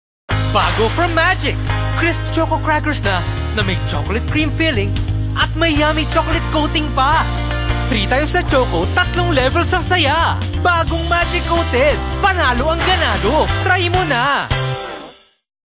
Magic Creams Coated TV15s
My 2nd "major" TVC VO gig. The first one I did was more than 2 years ago!

Announcer: Tom Vinoya
Producer: Datu Gallaga
Audio Post: Digitrax Post Productions